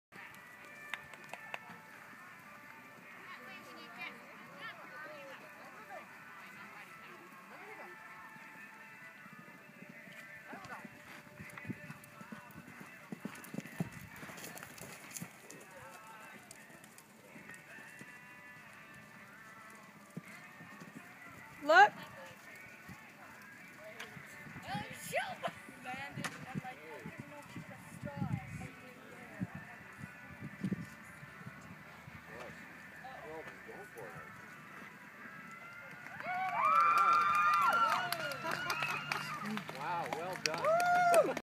The Jump off 2'9"
Mary's First Show On Cocoa Nut. After a frustrating day, forgetting courses and getting a refusal, she was determined to get a clear round and just nailed her classes on the last day. Great way to end the show!!! She was going so fast the Equine Canada medics asked to do a drug test on Cocoa. ha ha